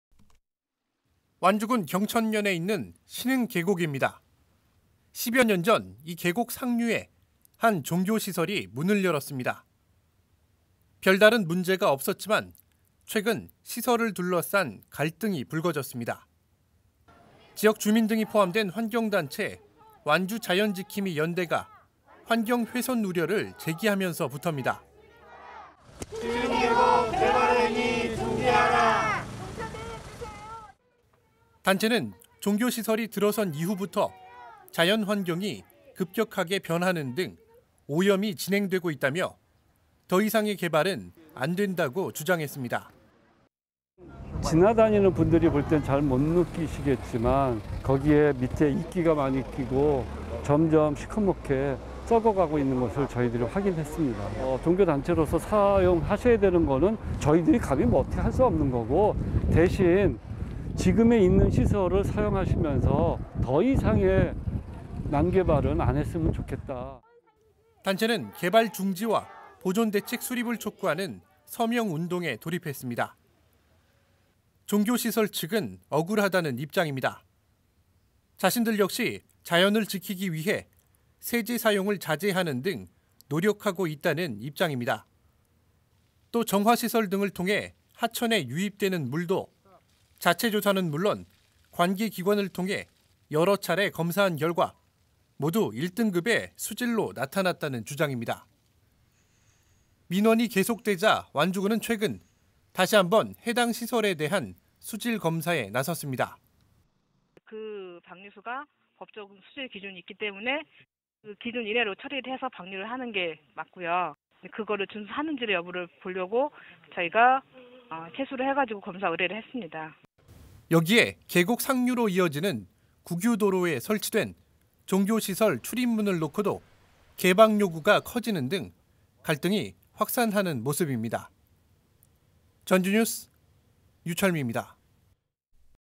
"개발행위 중지"vs"무리한 억측", 계곡 둘러싼 갈등
완주군 경천면 신흥계곡의 환경 오염문제로 지역 내 환경단체와 종교시설 간 갈등이 깊어지고 있습니다.